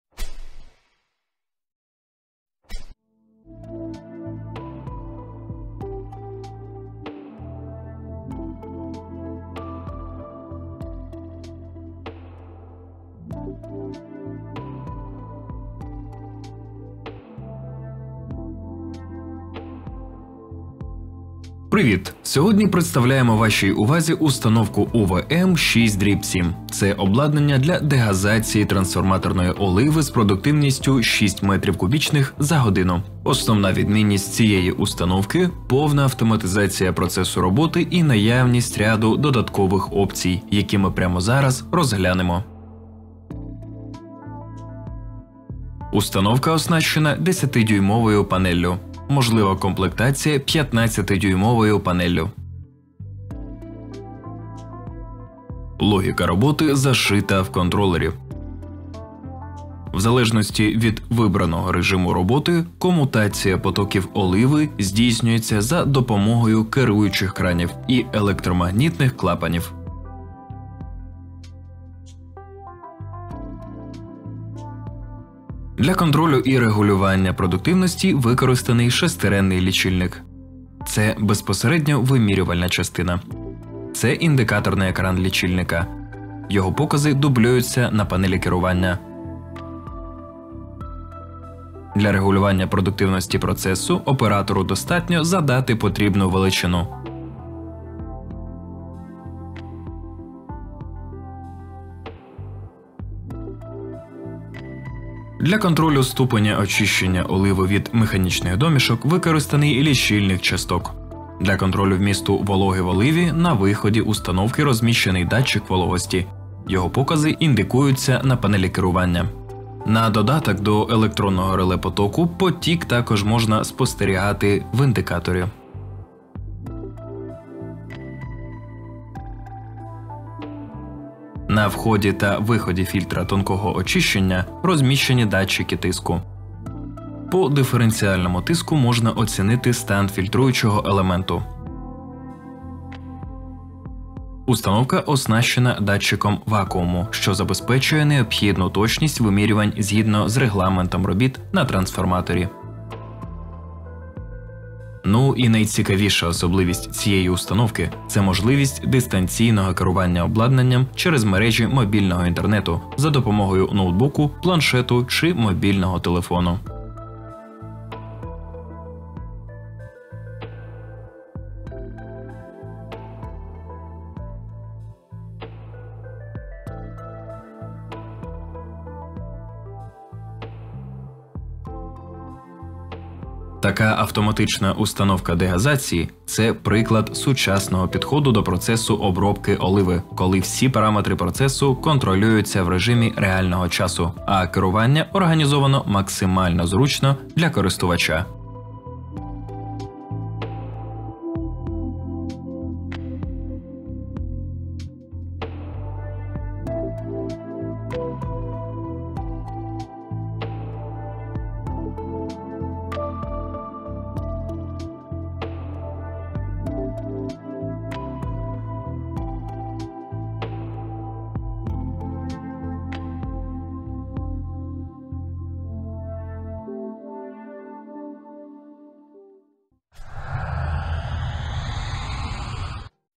УВМ-6/7 з управлінням touch-screen від GlobeCore
Дегазайійна установка УВМ-6/7 - це сучасне обладнання для дегазації трансформаторної оливи з автоматизованим процесом роботи та низкою опцій. Здатна до обробки оливи з продуктивністю 6 м³/год, вона використовує шестеренний лічильник для регулювання продуктивності та має додаткові функції контролю чистоти оливи. Найцікавішою особливістю є можливість дистанційного керування через мережі мобільного інтернету.

For all questions, Vitaliy Kolesnik
GlobeCore in Ukraine, "Plant Ukrbudmash", Poltava